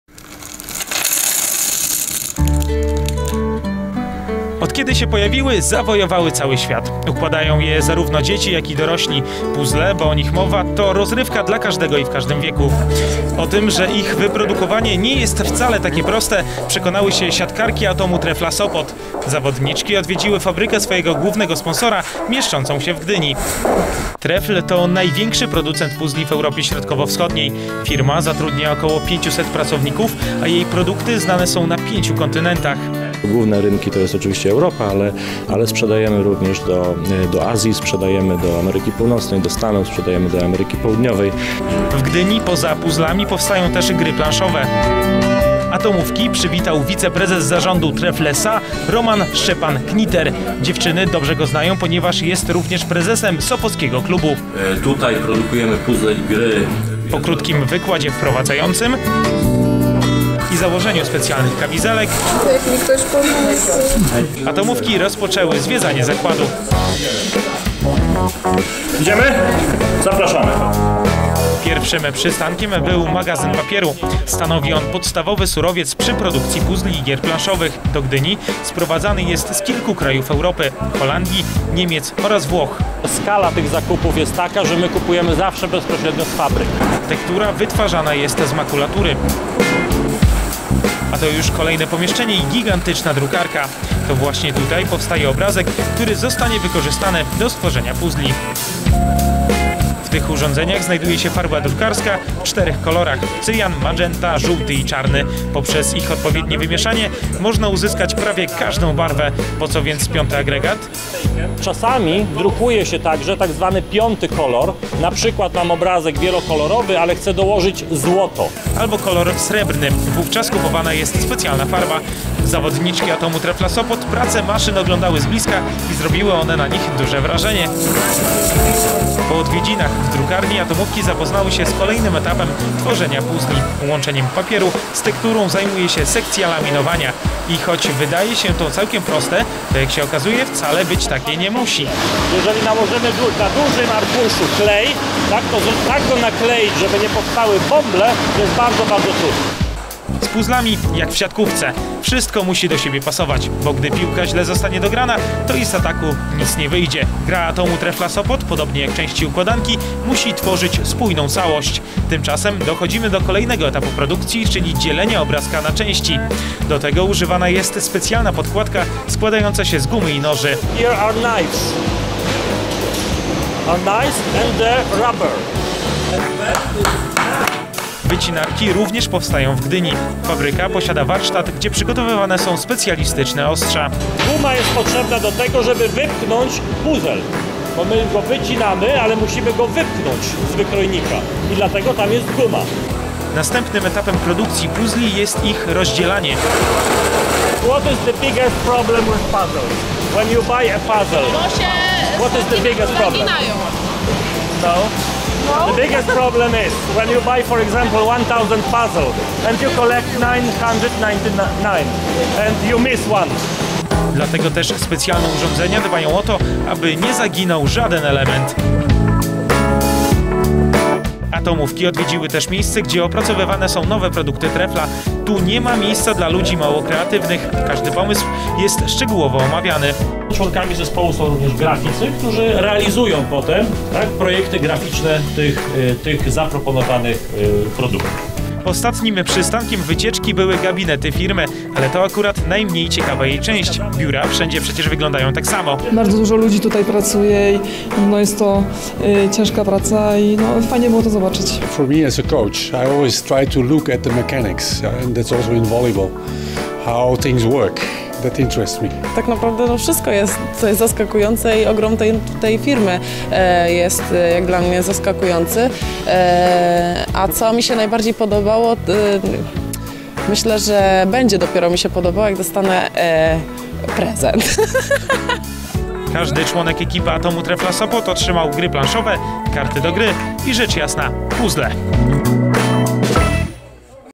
Atomówki odwiedziły fabrykę Trefla
Zrobienie dobrych puzzli nie jest wcale takie proste. Przekonały się o tym Mistrzynie Polski podczas wizyty w fabryce największego w Europie Środkowo-Wschodniej producenta puzzli firmy Trefl SA